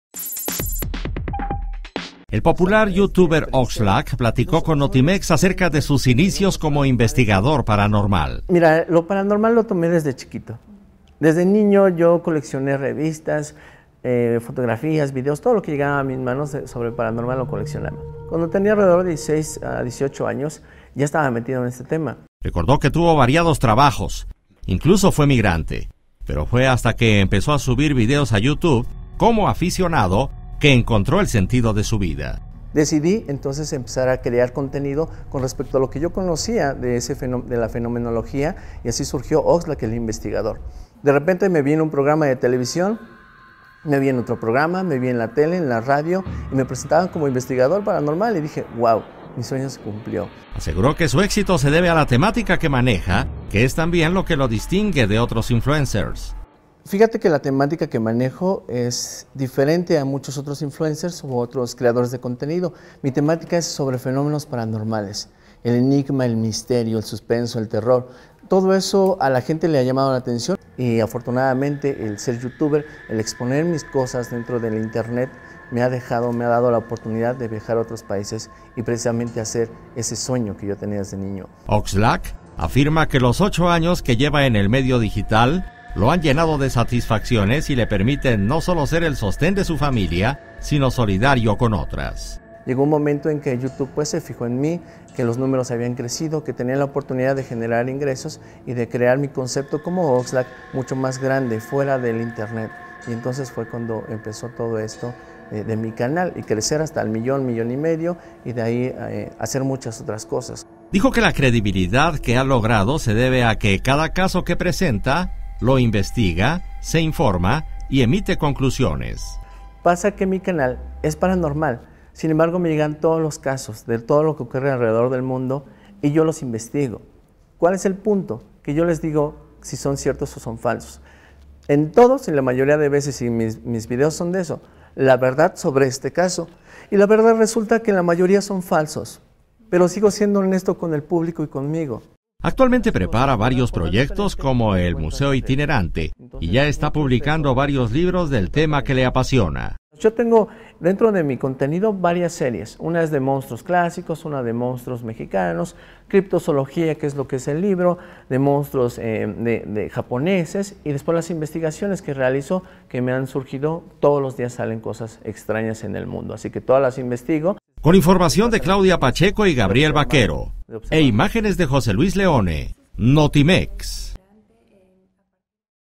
Oxlack: investigador de fenómenos paranormales e influencer
Uno de los youtubers más exitosos de la red donde expone,explica y emite conclusiones sobre casos de misterio y energía ocultas